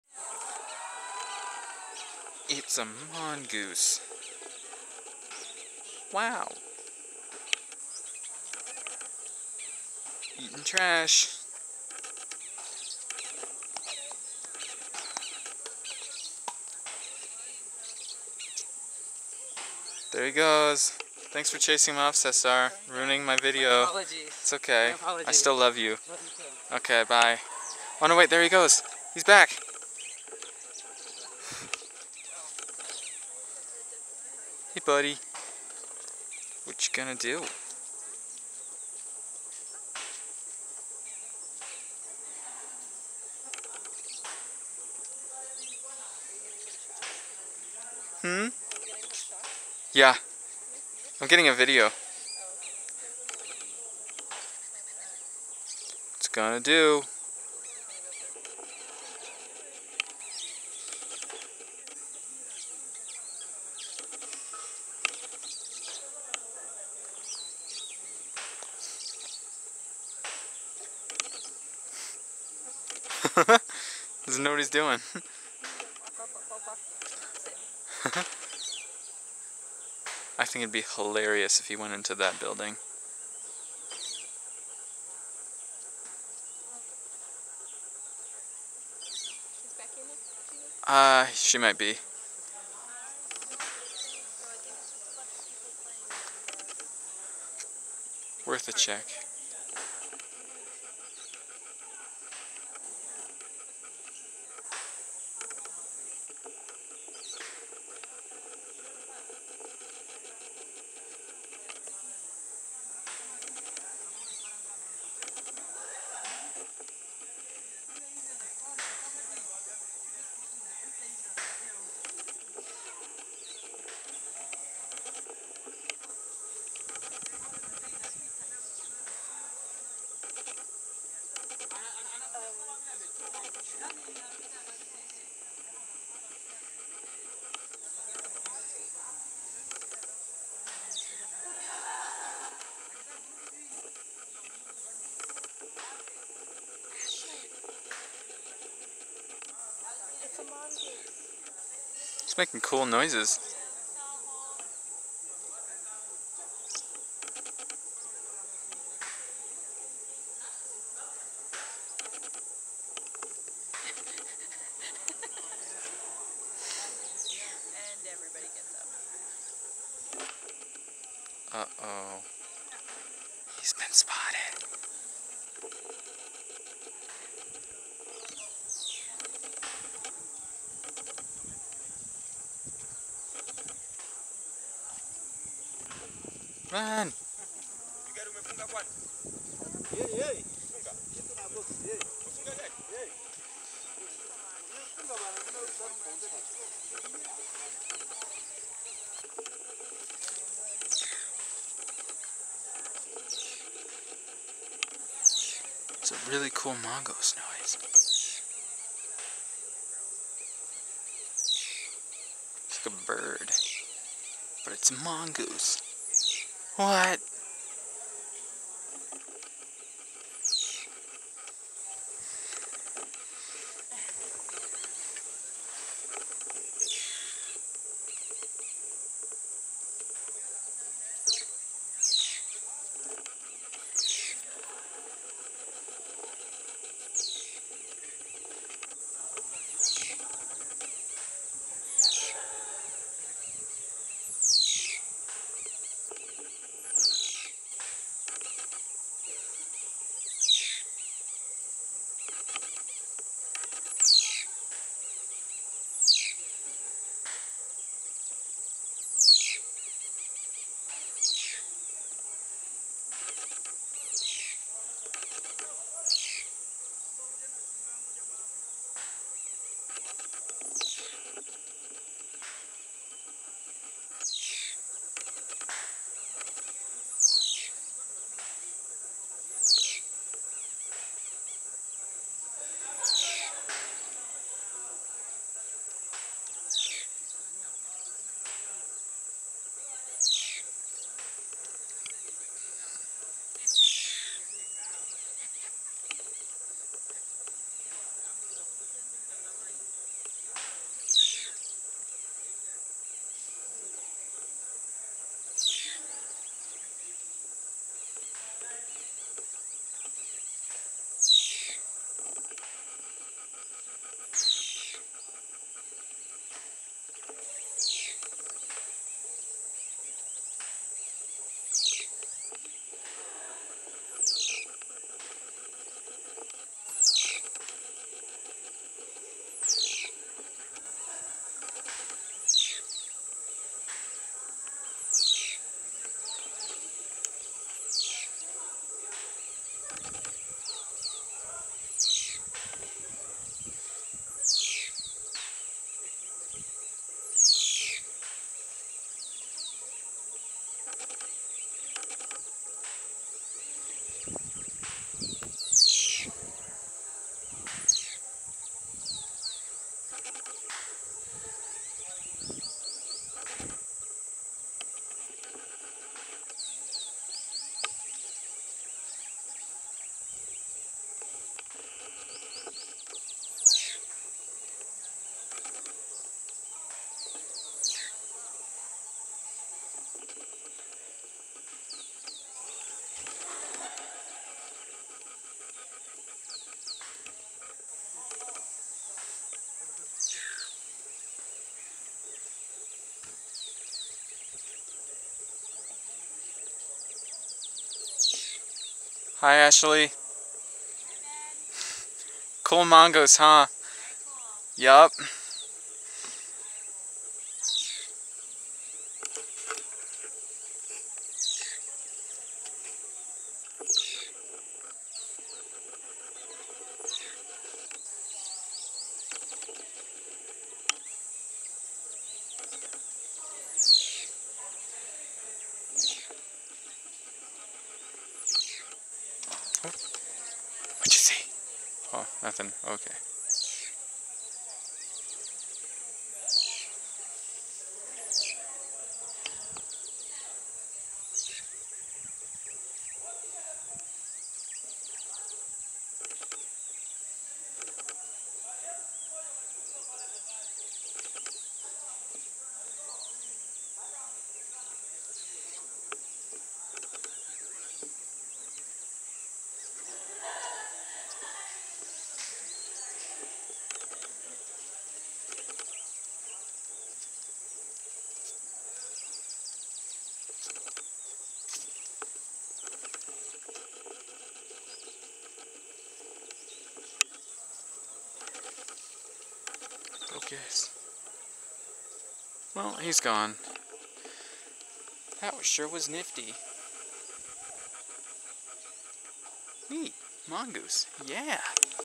A mongoose in the camp
Gee willikers a banded mongoose in the camp! Get the camera!